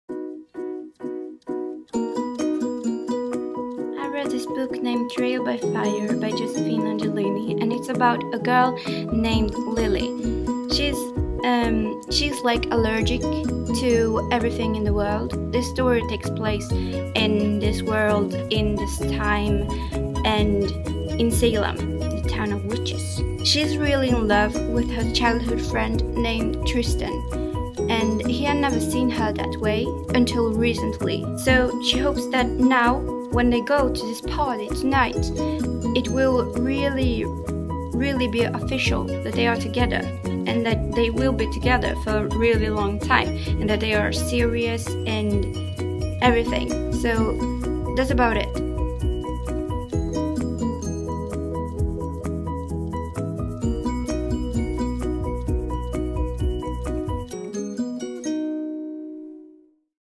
Trail by fier by Josephine Angelini TRAIL BY FIRE
Den här videon handlar om Trail by fier by Josephine Angelini
This is my fIrst public video sorry for picture and sound I filmed it with my webbcam